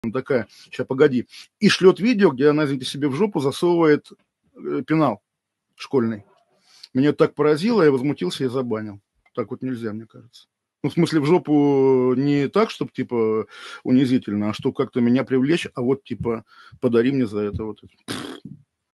Олег Кашин и неожиданное видео
Олег Кашин рассказал как поступать, если женщина проявляет чересчур откровенные знаки внимания
уникальный канал с видеоконтентом, где в главных ролях узнаваемые персонажи!